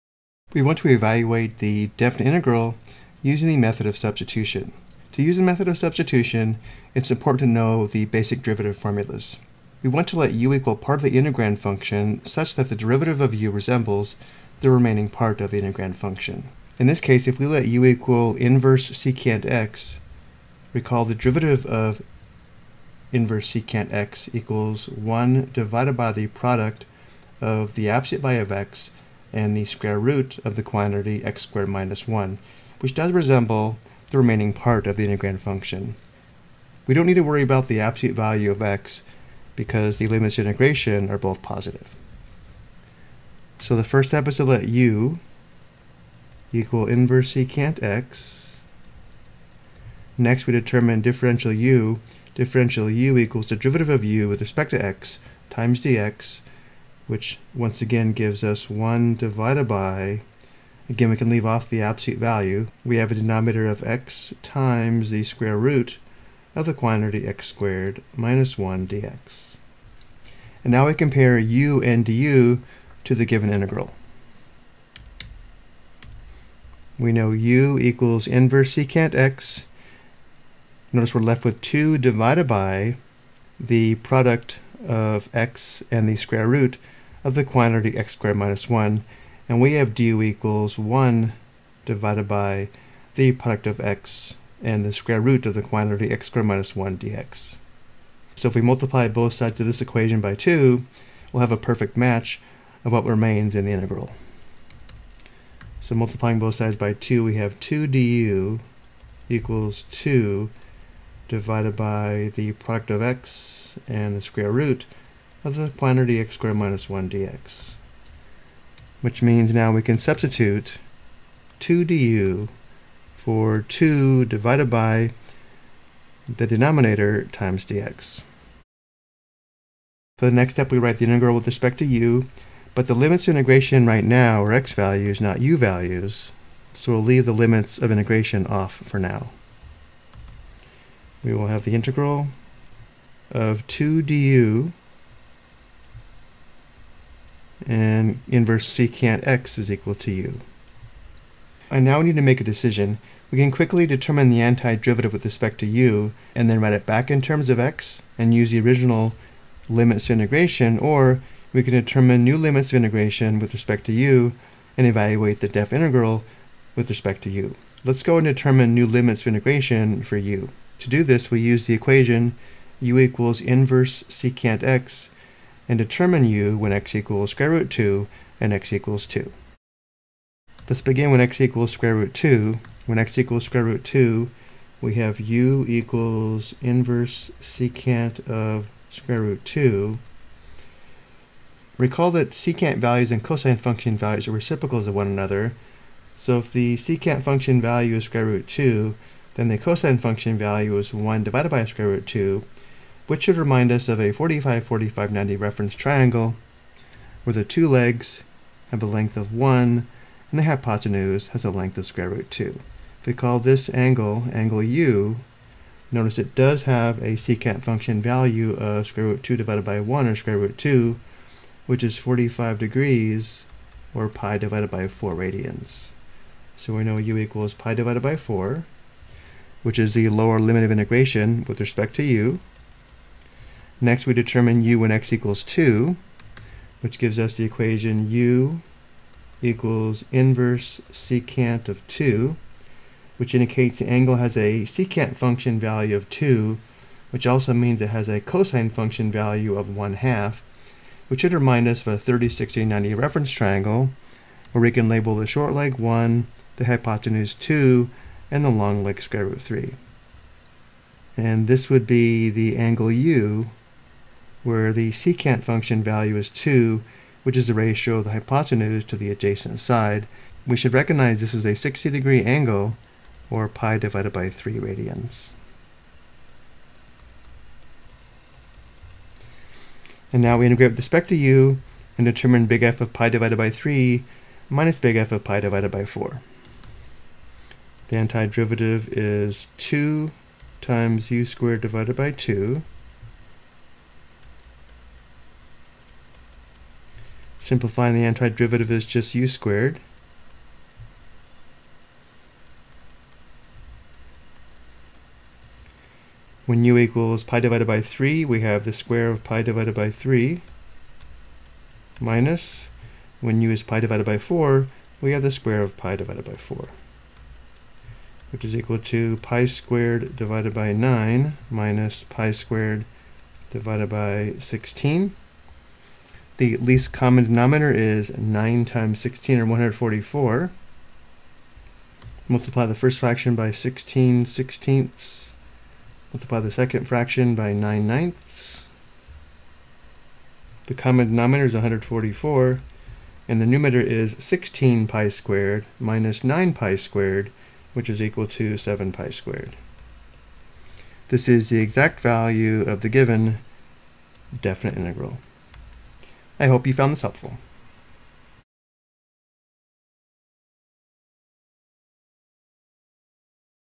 Definite Integration Using Substitution: Int(2arcsec(x)/(x*sqrt(x^2-1,x,sqrt(2),2)
This video provides an example on how to evaluate a definite integral using substitution.